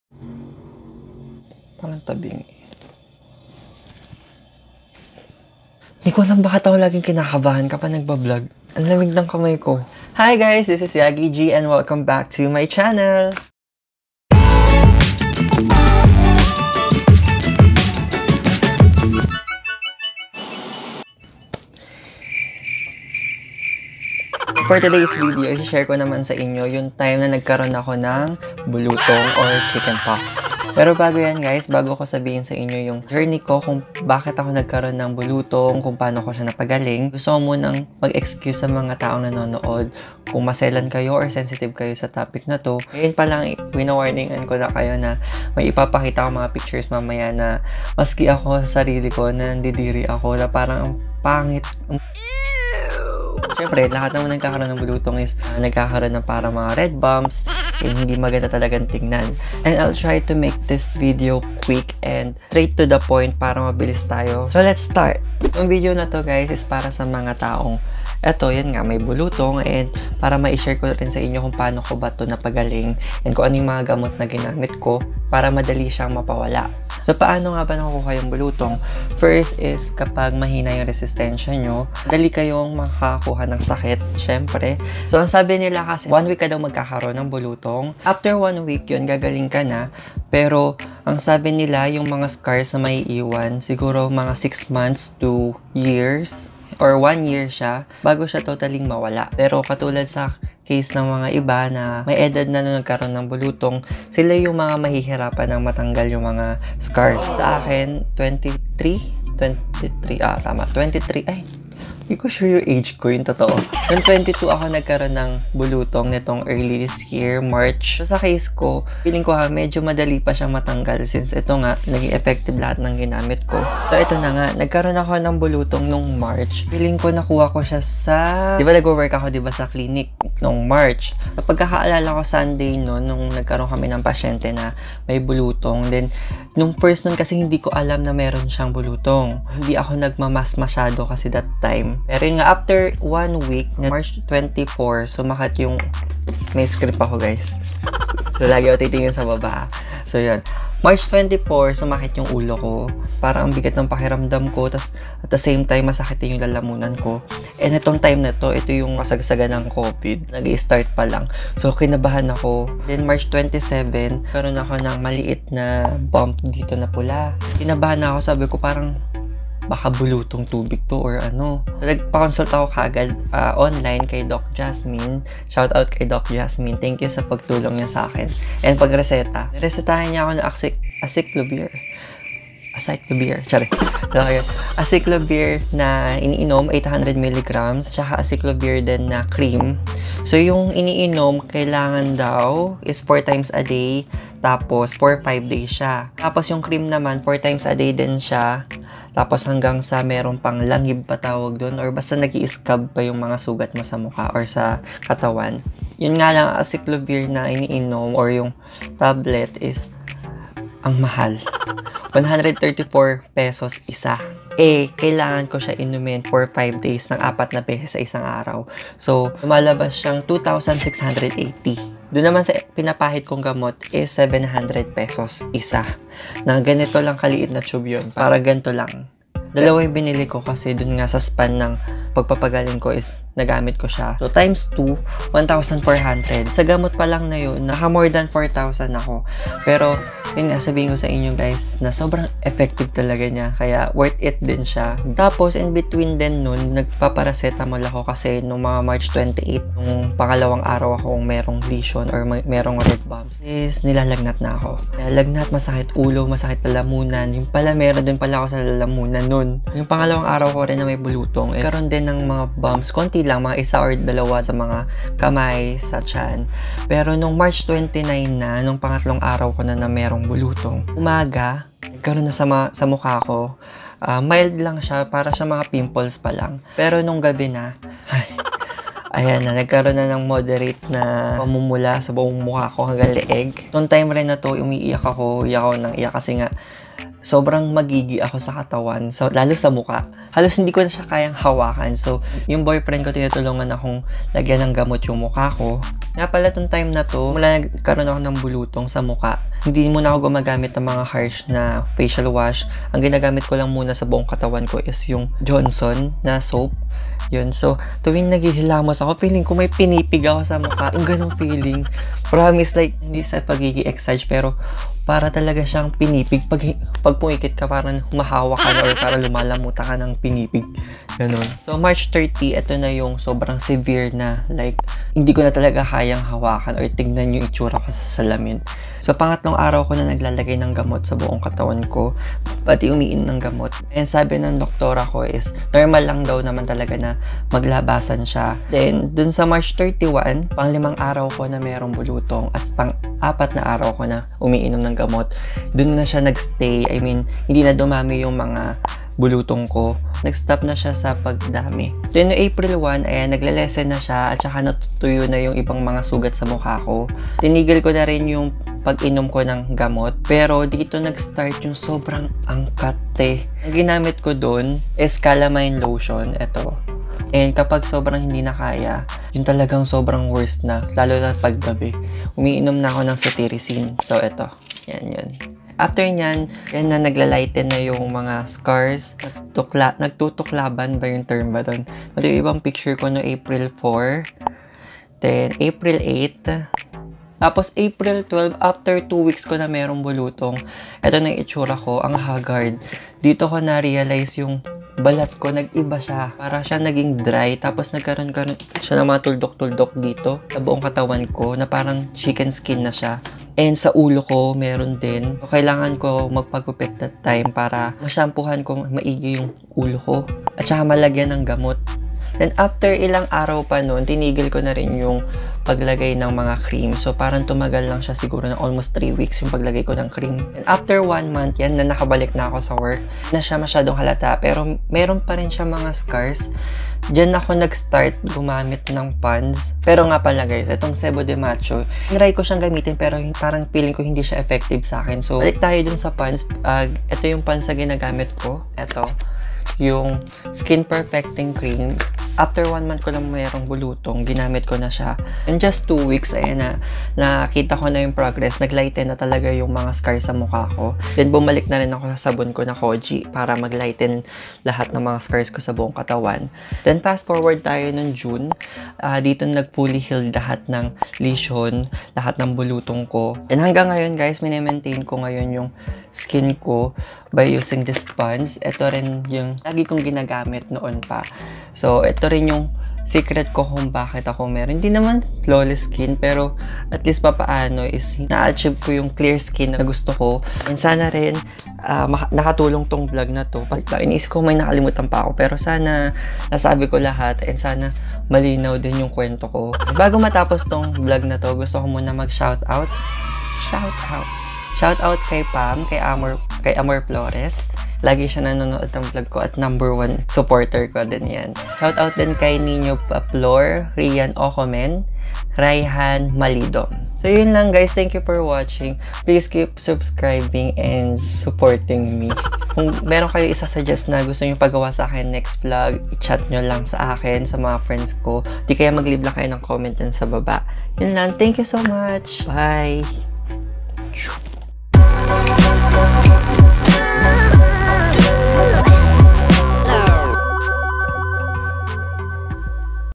How to get clear skin + how to get rid of CHICKEN POX scars ft. POND'S SKIN PERFECTING CREAM DETOX
Sharing my Chicken Pox journey 🙂